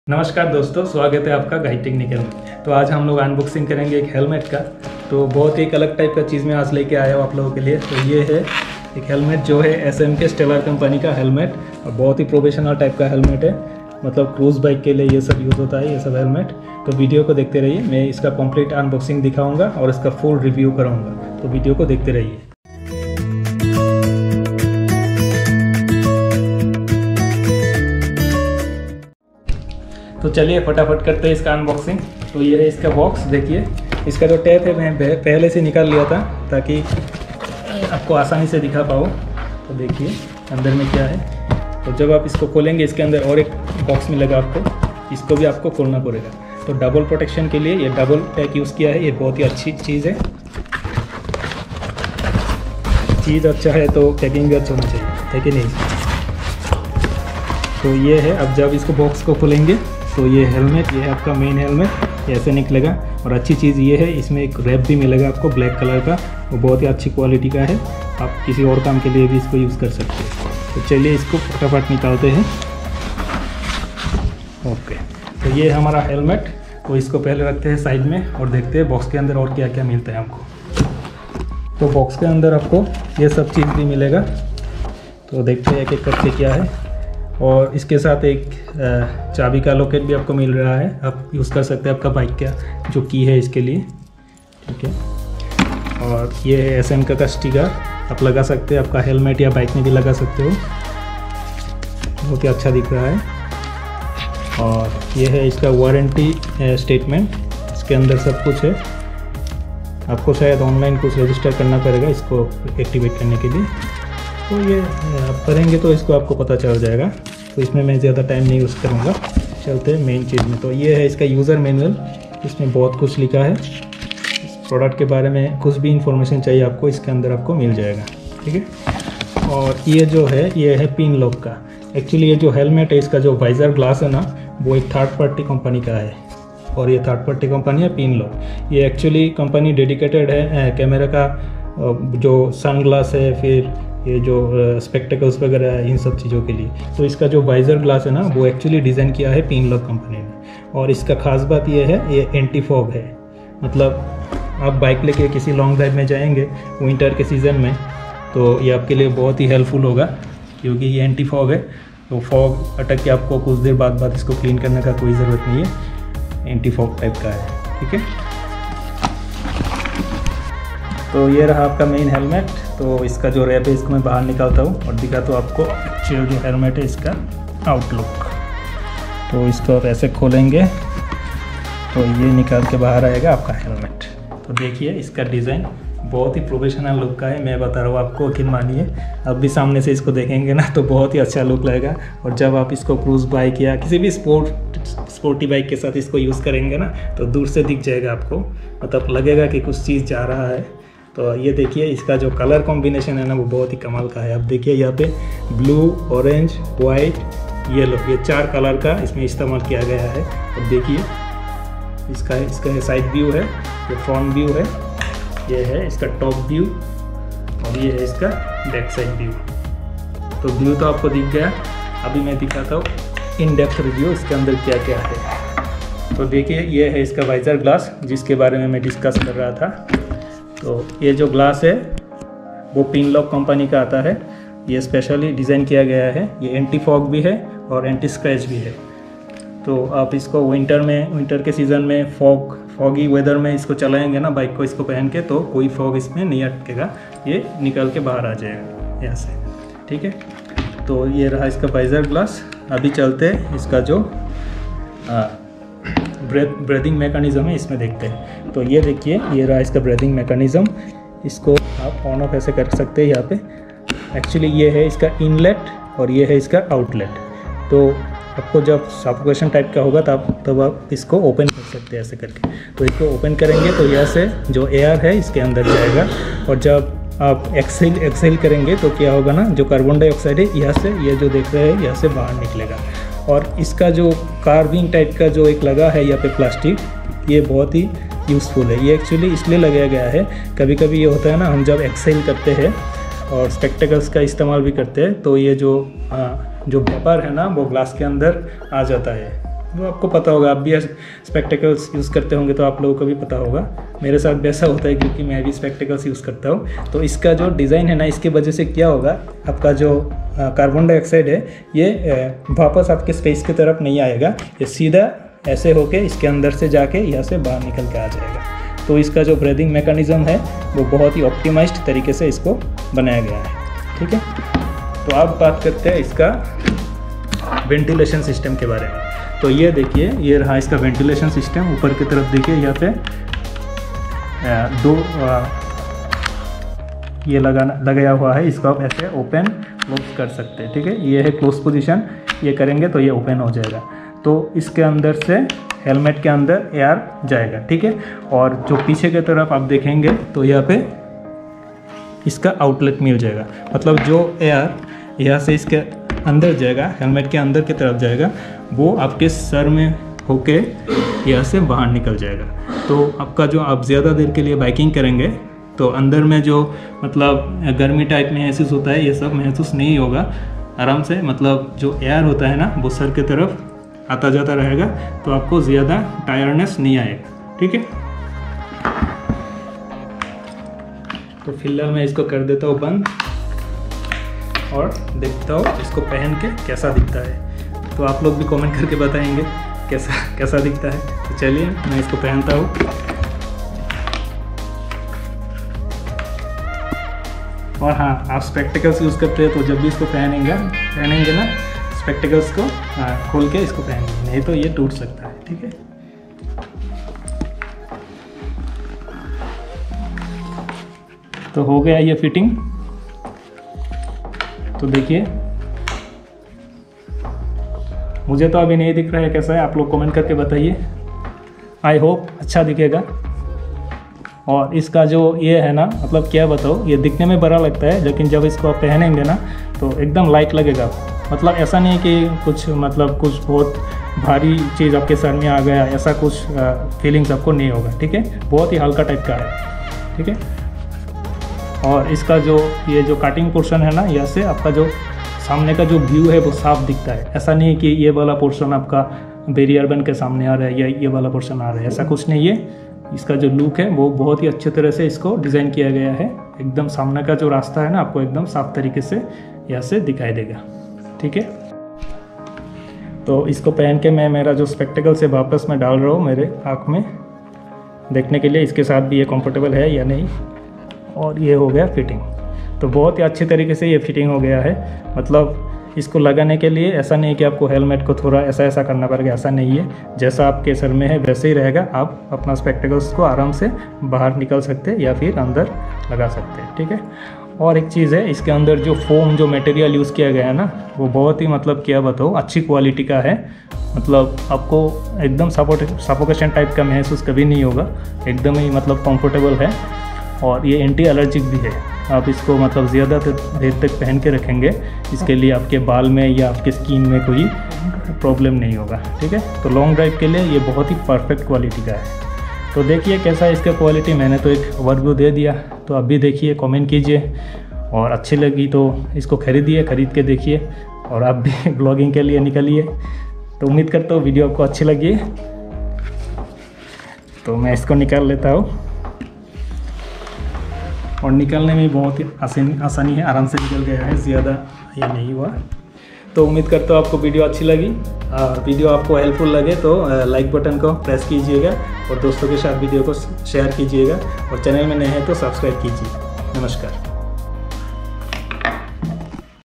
SMK Stellar Helmet with Pinlock Anti-Fog Glass || Perfect for Motovlogging || Unboxing & Review
If you are looking for a professional look anti-fog helmet for your cruise bike then this video is made for you. In this video I have shown unboxing and detailed review of SMK STELLAR ANTI-FOG HELMET which is specially designed for motovlogging. Watch the video till the end and share your own thoughts on this product in comment section.